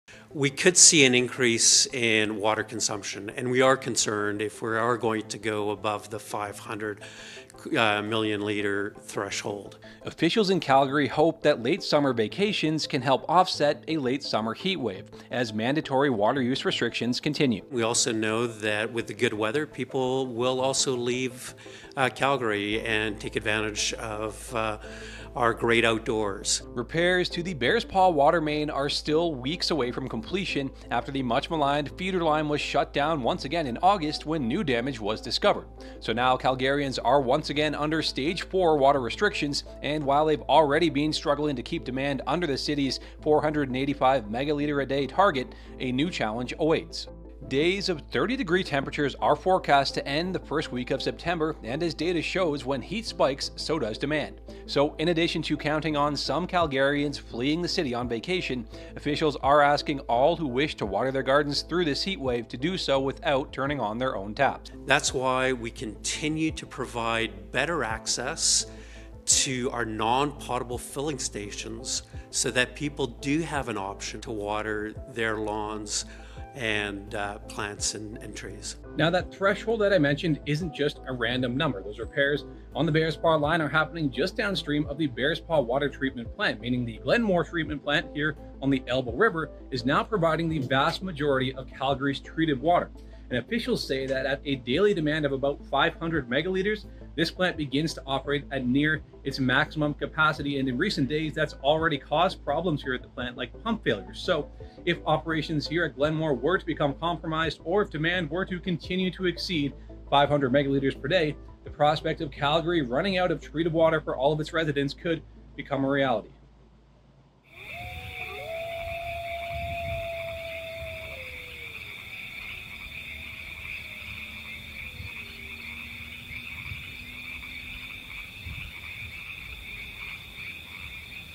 Heat Wave Puts Pressure on Calgary to Better Conserve Water
With work still being done on water mains in Calgary, the city is asking people to do more to stay under a critical water usage threshold. Connor O'Donovan explains what may happen if the heat in the city pushes water usage past that limit.